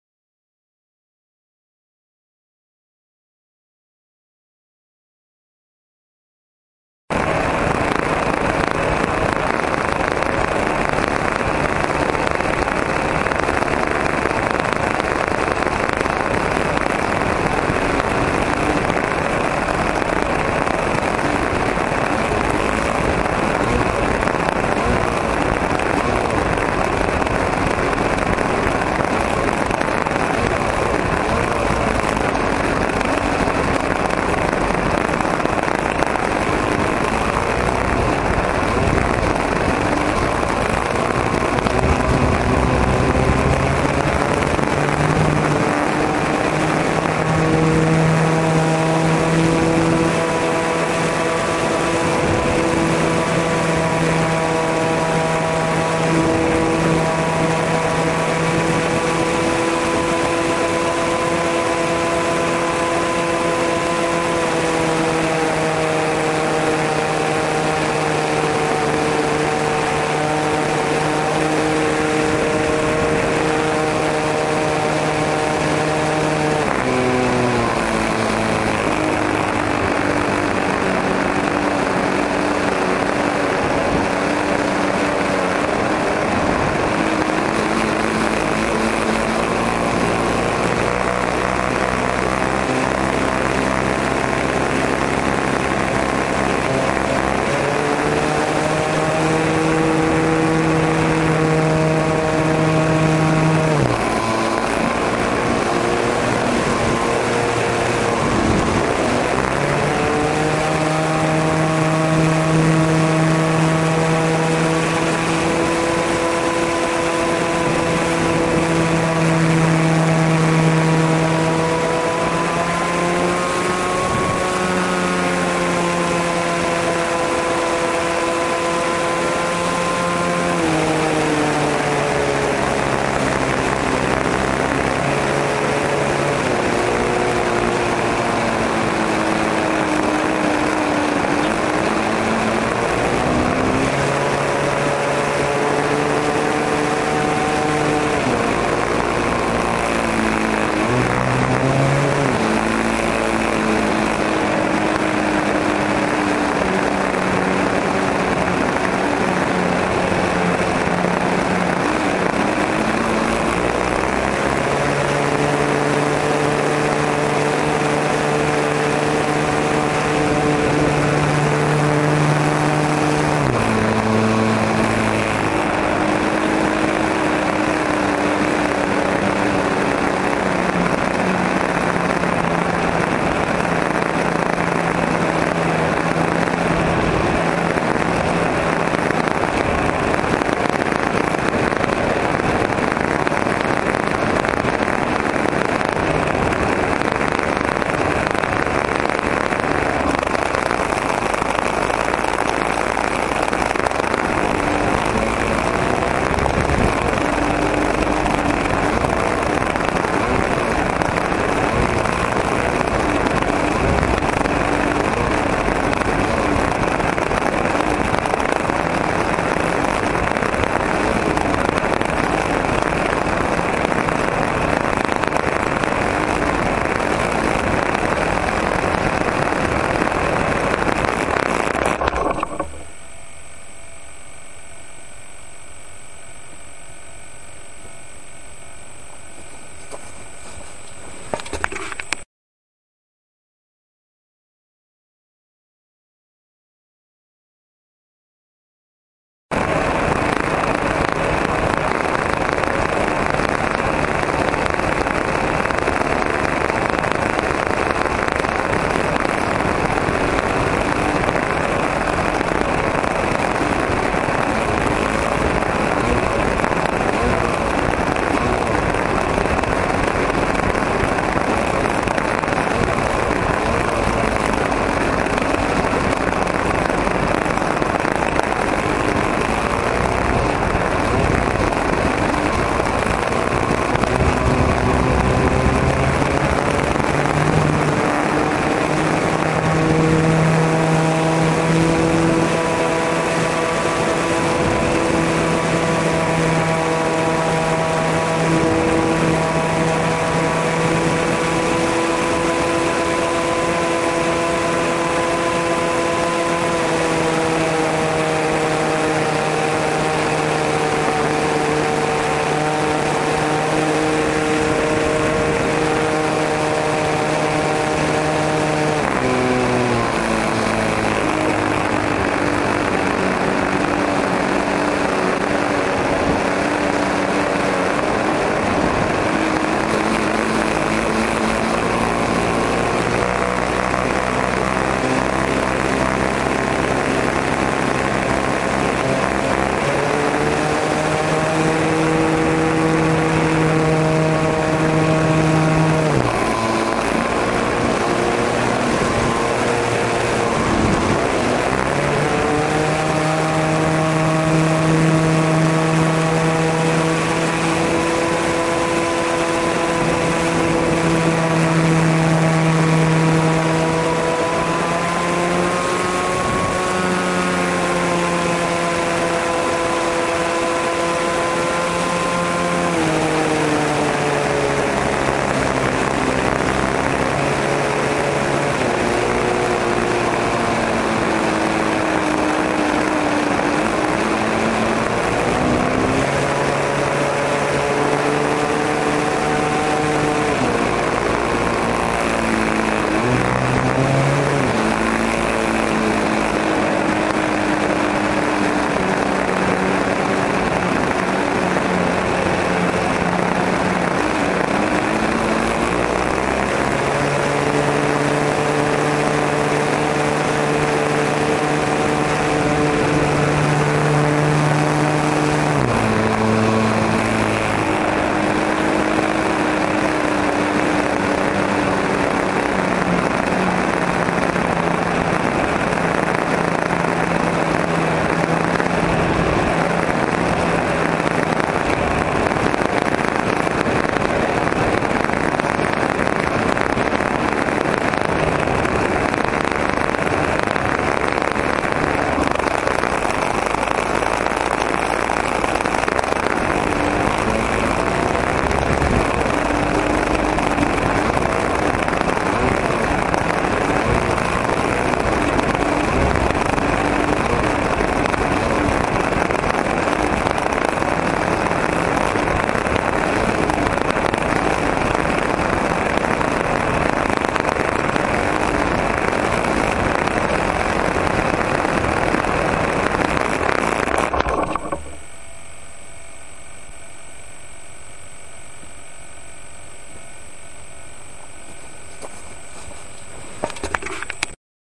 Calmato Sport 40
Volo con Calmato sport 40 Fly Cam One HD fissata sotto. Motore os 40 ,Dualsky -  Regolatore di tensione VR-5L 5A una batteria Robbe -  Lipo RX 1500 mAh 2S .